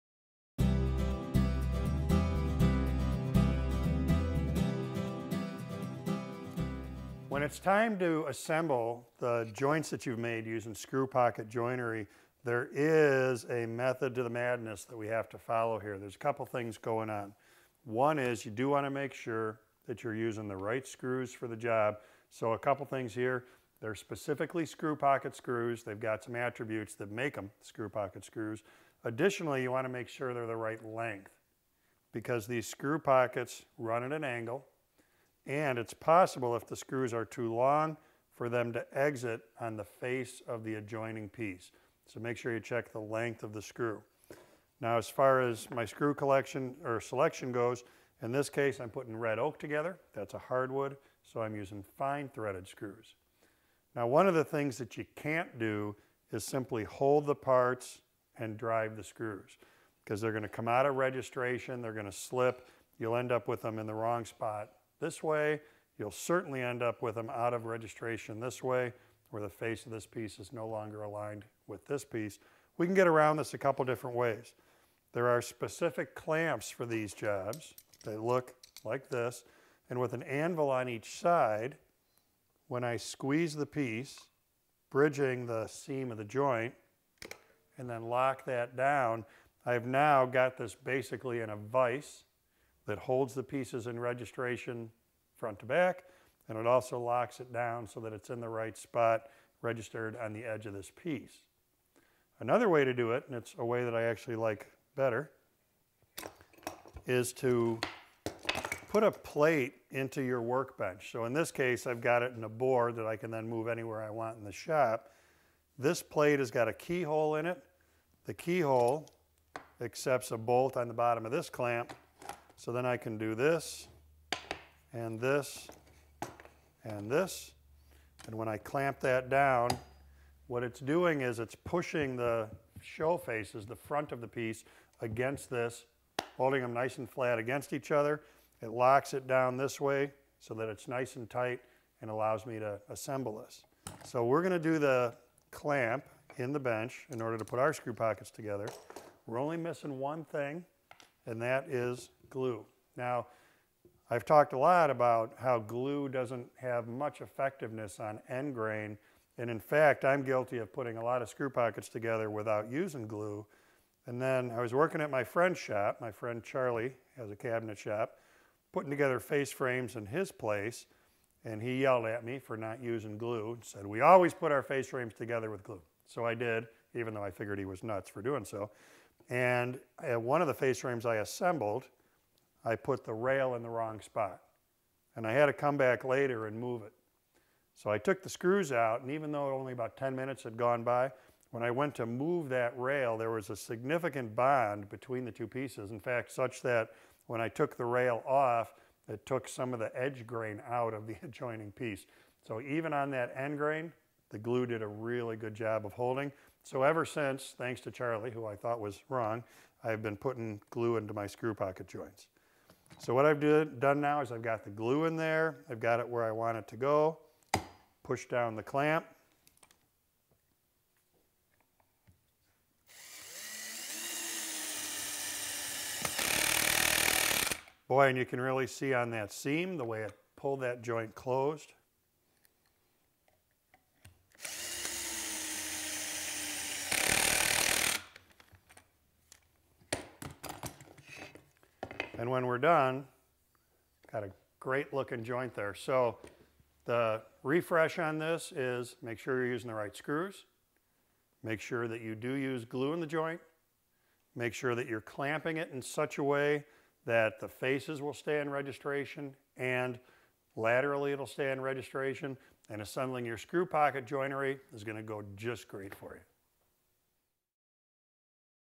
What’s the Deal with Screw Pocket Joinery | Woodworkers Guild of America
Sign up for the free WWGOA newsletter to receive the latest videos, articles, and tips straight from the experts!

Screw pockets can be used to create a solid structure on a number of woodworking projects. When fastening screw pocket joinery, you’ll want to pay attention to a few key details, including the type and length of your screws and the method of clamping that you use.